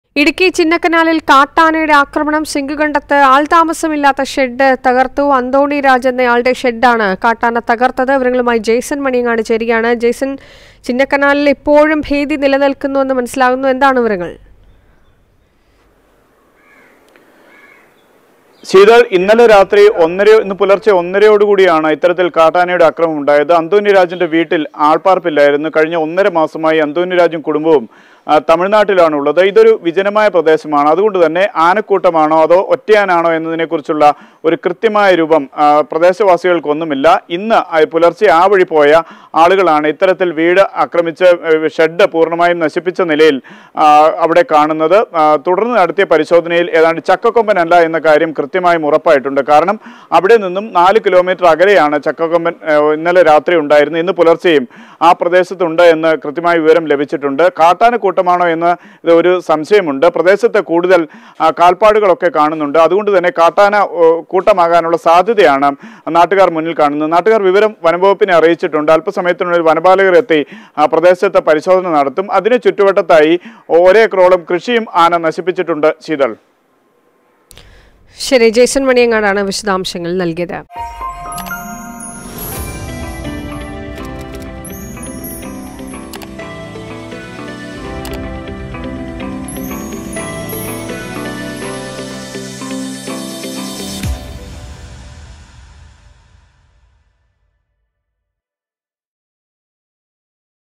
ഇടുക്കി ചിന്നക്കനാലിൽ കാട്ടാനയുടെ ആക്രമണം| Chinnakkanal| Wild elephant| Idukki
ഇടുക്കി ചിന്നക്കനാലിൽ കാട്ടാനയുടെ ആക്രമണം; സിങ്കുകണ്ടത്ത് ആൾതാമസമില്ലാത്ത ഷെഡ് തകർത്തു
 Chinnakkanal| Wild elephant| Idukki